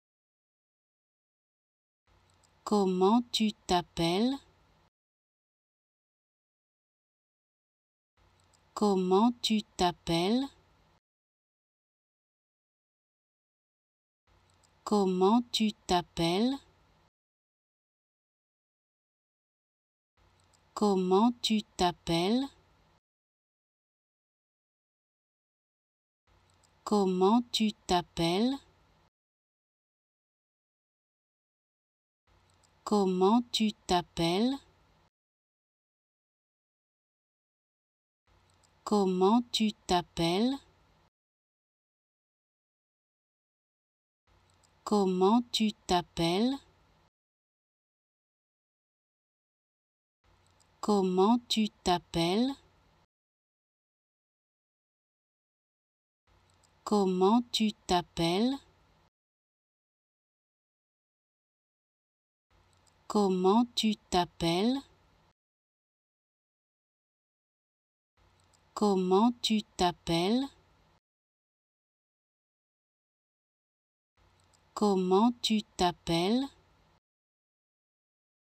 How to Pronounce ''Comment tu t'appelles'' (What's your name?) Correctly in French
Learn how to say and properly pronounce ''Comment tu t'appelles'' in French with this free pronunciation tutorial. Apprenez à prononcer des mots en français grâce à nos tutoriels.